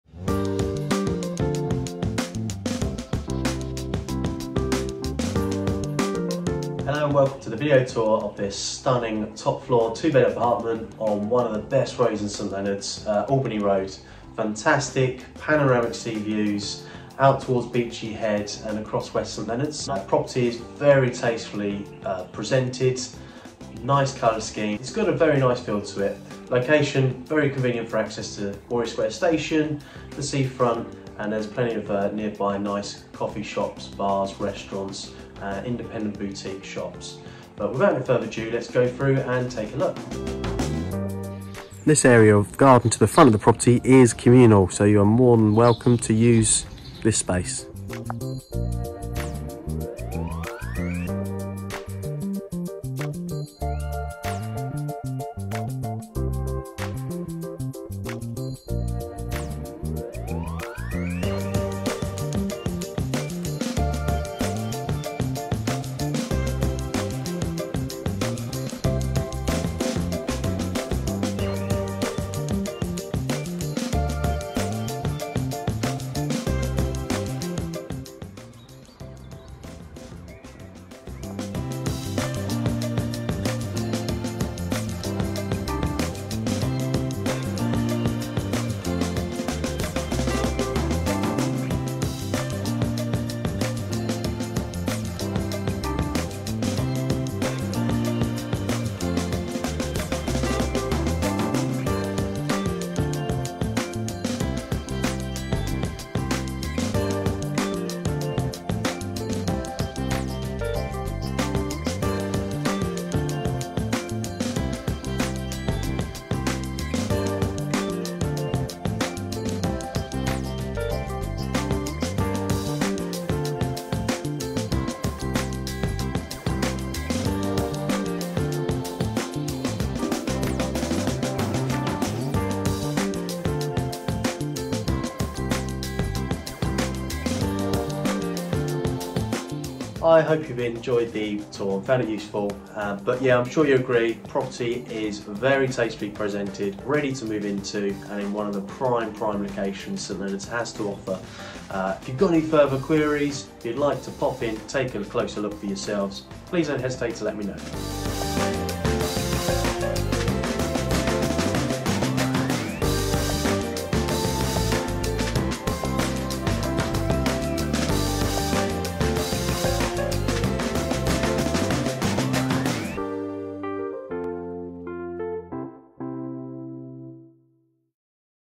forsale Albany Road
A two bedroom top floor apartment, situated in one of St. Leonards most sought after tree lined streets with sea views.

To arrange a viewing get in touch on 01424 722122.